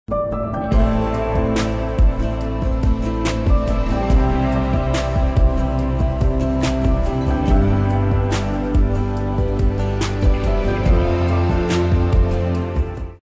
Unleash Your Creativity with Sony's α6700
With Sony's α6700, creators can record videos up to 4K60P in 10:2:2 to create cinematic expression and elevate the video.